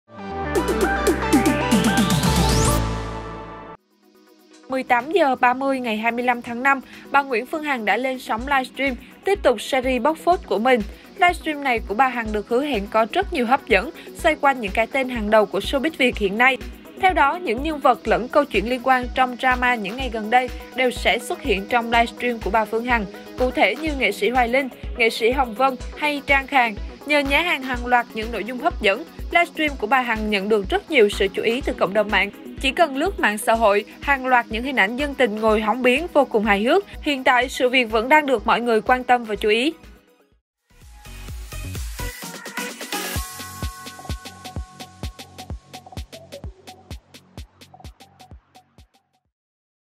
Người người, nhà nhà hóng livestream nữ đại gia Phương Hằng
. Tối 25/05, livestream của bà Phương Hằng nhận được nhiều sự chú ý từ cộng đồng mạng. Chỉ cần lướt mạng xã hội hàng loạt hình ảnh dân tình ngồi "hóng biến" vô cùng hài hước.